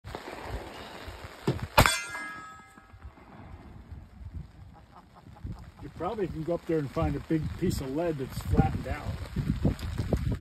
Remington Rolling Block Rifle
Shooting an old Remington rolling block rifle.